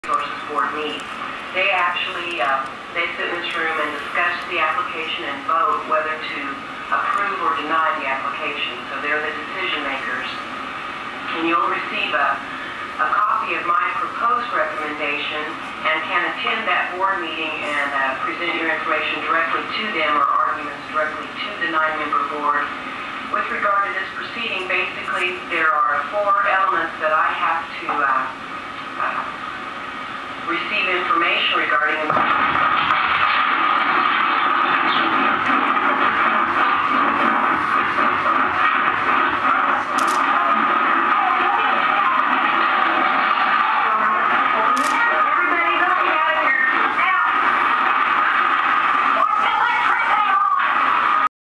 OKC Bombing Museum
A reenactment of the moment of the bombing, set in a nearby courtroom.